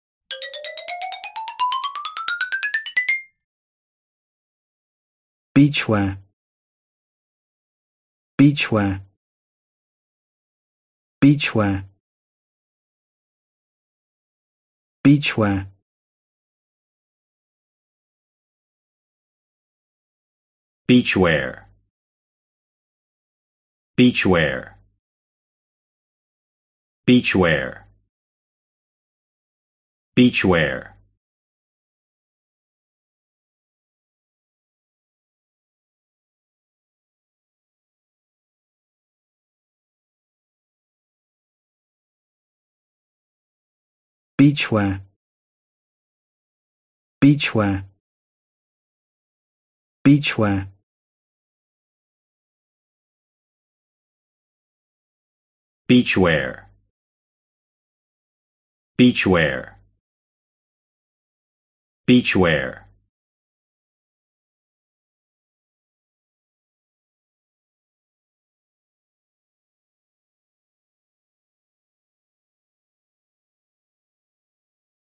英単語 beachwear 発音と読み方
英単語beachwearの発音動画です。

イギリス英語での発音記号は「ˈbiːtʃ.weər」でアメリカ英語での発音記号は「-wer」です。